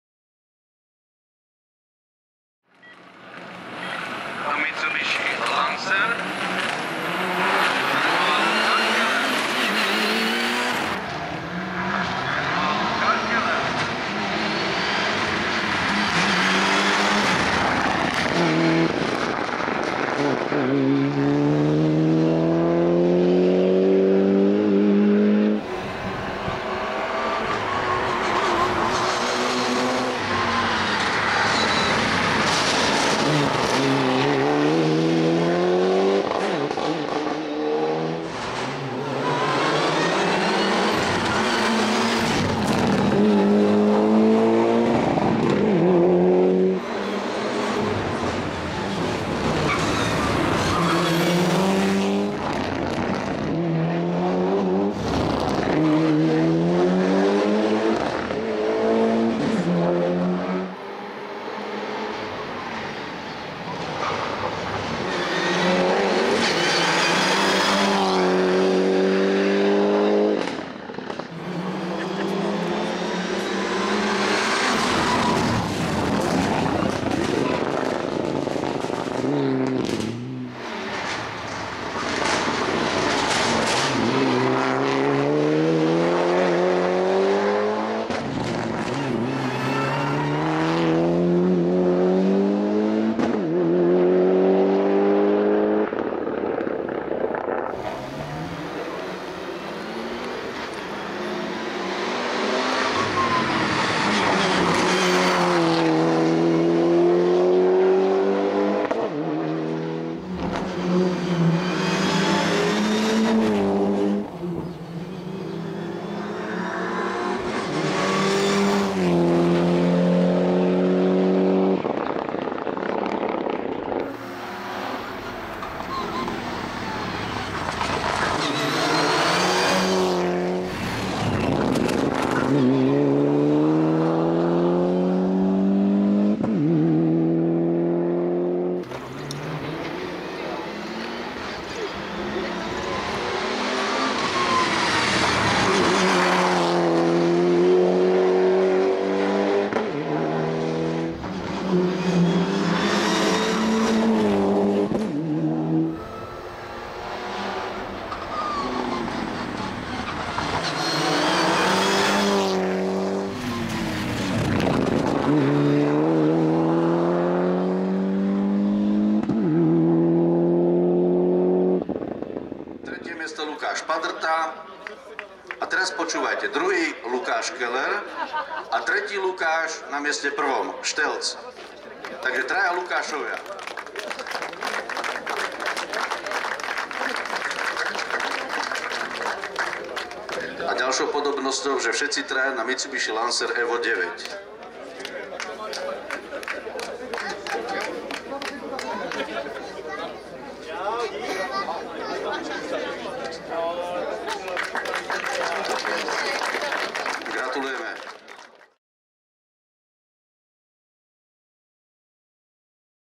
Lukáš Keller - Mitsubishi Lancer Evo IX - XXXX. Zámecký vrch 2021 - Náměšť nad Oslavou
Lukáš Keller si s vozem Mitsubishi Lancer Evo IX dojel v sobotu pro druhé místo ve třídě S1+2000/4WD v závodech do vrchu XXXX. Zámecký vrch 2021 v Náměšti nad Oslavou, a v neděli svou třídu vyhrál.